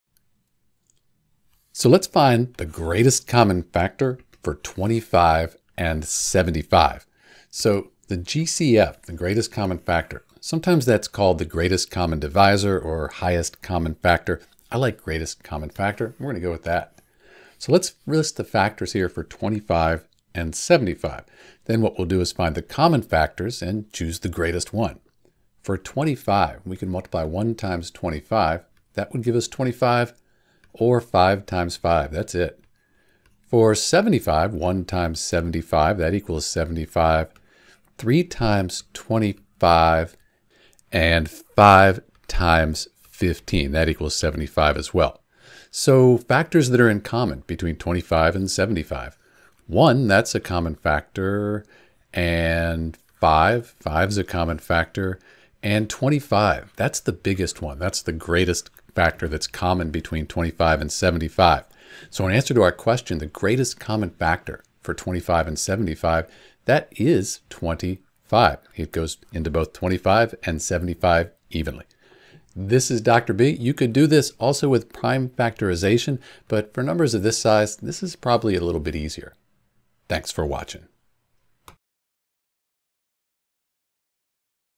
How to Find the Greatest Common Factor for 25 and 75
• The factors of 25 are the numbers that can evenly divide 25. In this case, they are 1, 5, and 25.

• The factors of 75 are the numbers that can evenly divide 75. In this case, they are 1, 3, 5, 15, 25, and 75.

Common Factors: 1, 5, 25

Note: You can also use prime factorization to find the GCF. It involves breaking down each number into its prime factors and then finding the common prime factors.